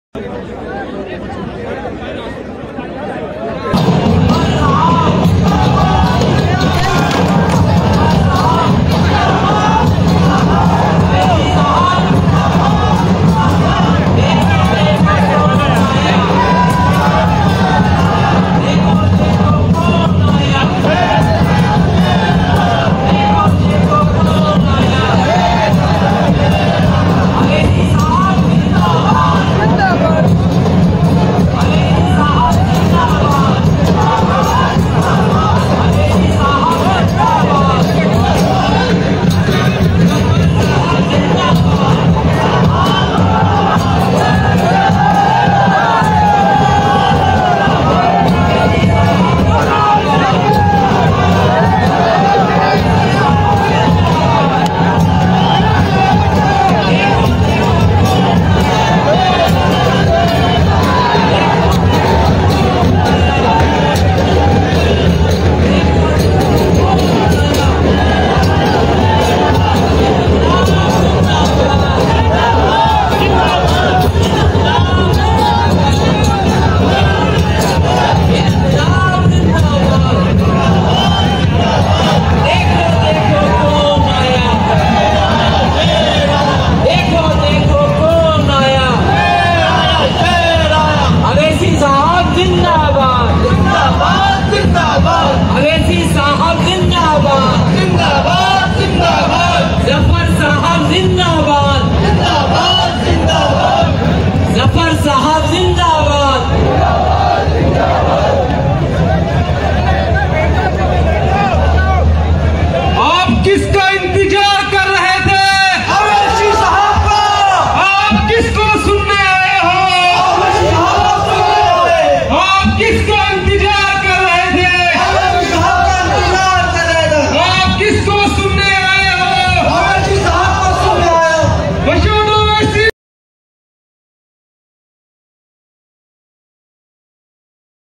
राजस्थान के सवाई माधोपुर में असदुद्दीन ओवैसी साहब की धमाकेदार एंट्री
Asaduddin Owaisi साहब ने आज Rajasthan के SawaiMadhopur में विशाल चुनावी जनसभा को संबोधित किया।